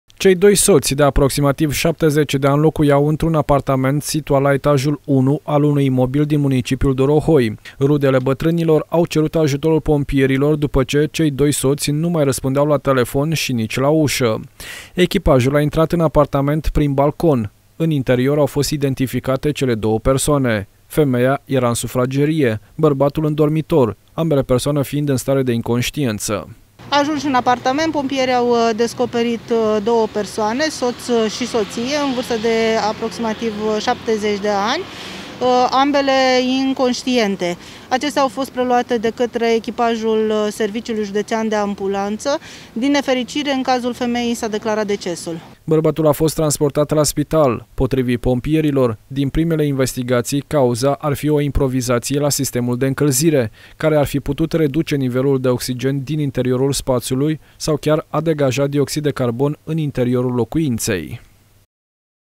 BATRANI GASITI INCONSTIENTI IN CASA. O FEMEIE A MURIT
Descoperire șocantă într-un apartament din Dorohoi. Pompieriichemați să intervină pentru deblocare ușii apartamentului în care locuieu doi soți, iar găsit pe aceștia căzuți, în stare de inconștiență. Medicii de la Servicul Județean de Ambulanță i-au preluat pe cei doi bătrâni, însă din nefericire în cazul femeii au fost nevoiți să declare decesul. Cel mai probabil , cauza nenorocirii este o impovizatie la sistemul de încălzire, care ar fi putut reduce nivelul de oxigen din interiorul spațiului sau chiar a degajat dioxid de carbon în interiorul locuinței.